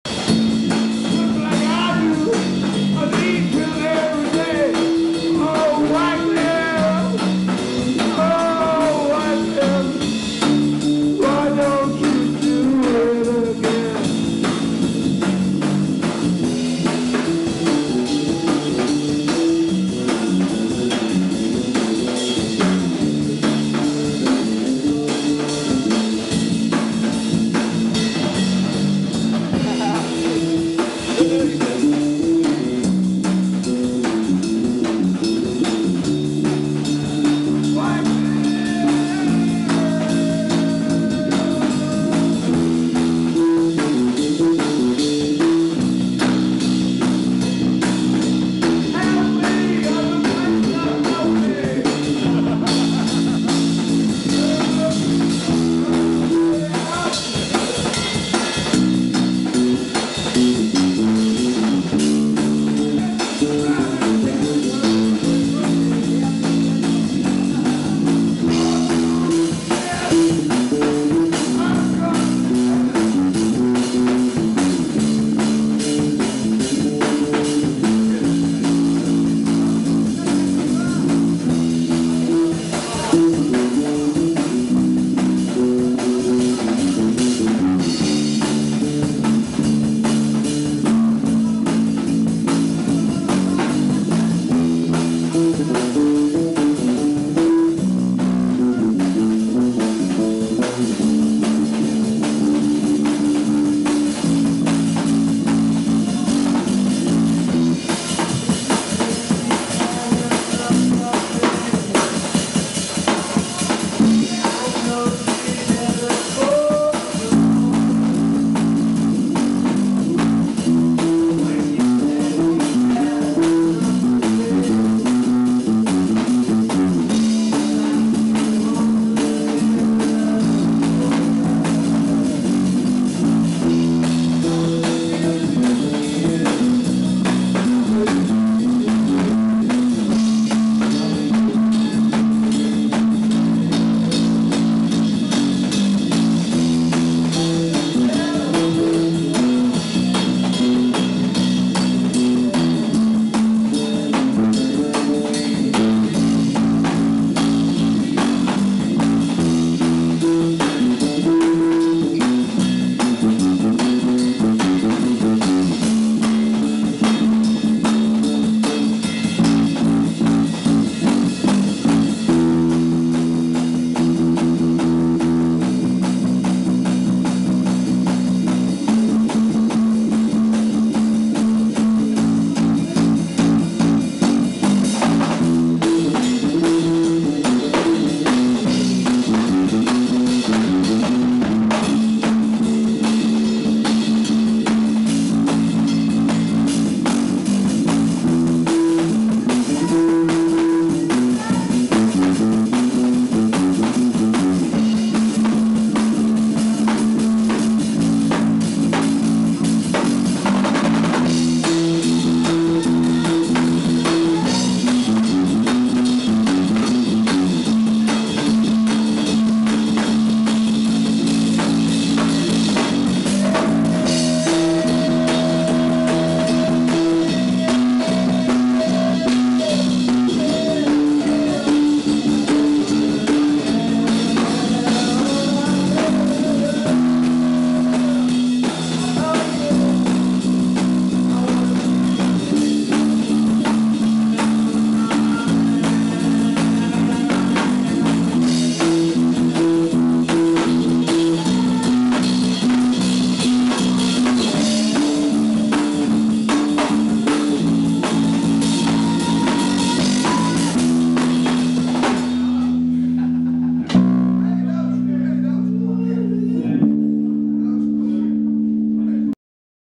Jammin at The Maze with David Silitoe(Bass)-James Waring(Drums)-Roxy Rob on vocals Hi-Def (00066)
,,,Jammin at The Maze with David Silitoe(Bass)-James Waring(Drums) and Roxy Rob on vocals...November 2010...